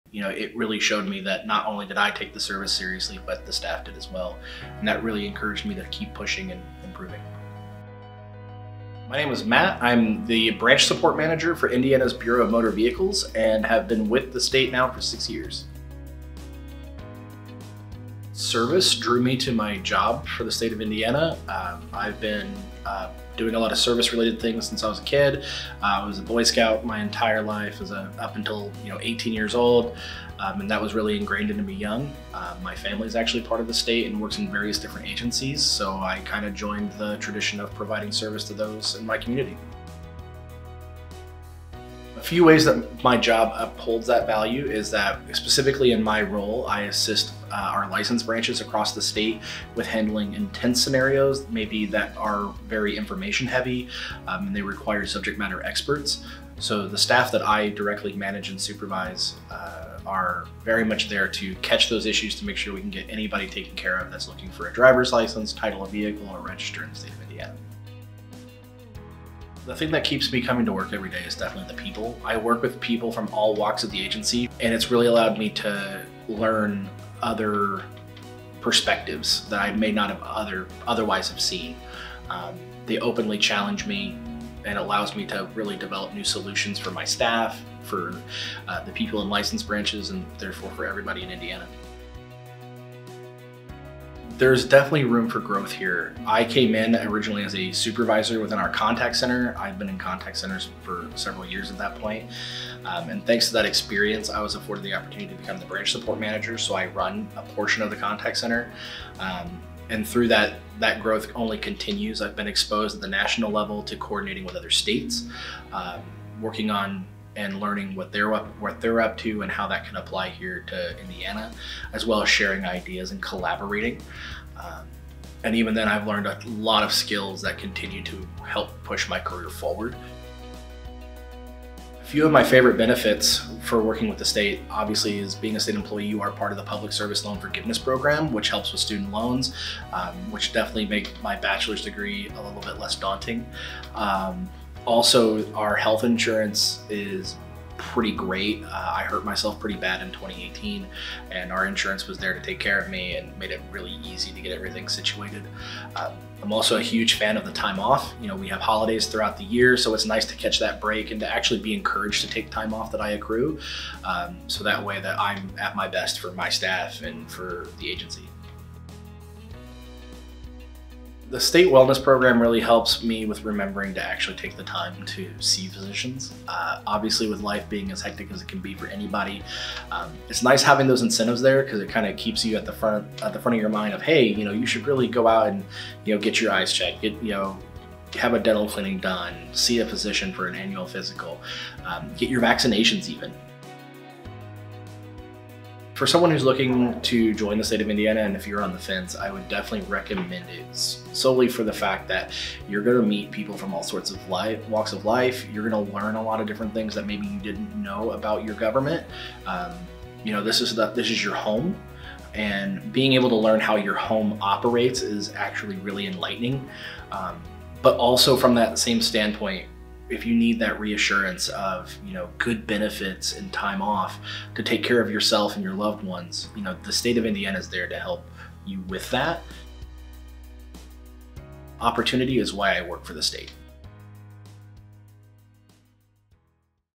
Why I Work for Indiana - BMV Branch Support Manager
Matt reflects on his role as Branch Support Manager for the Indiana Bureau of Motor Vehicles, and why he chooses to WorkforIndiana!